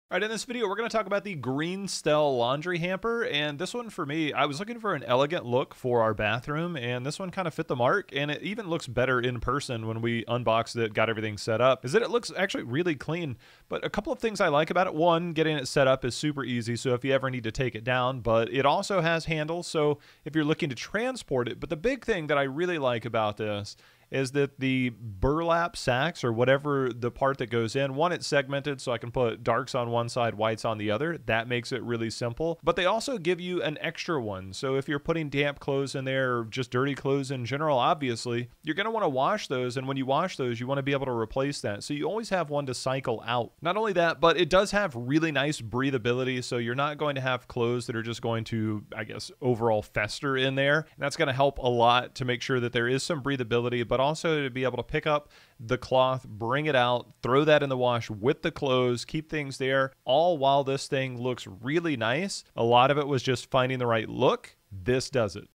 GREENSTELL Laundry Hamper Review
We were looking for something simple and elegant enough and we tried the GREENSTELL laundry hamper from Amazon and this is our review. It's durable, well made, and we really appreciate the extra bag that comes with it.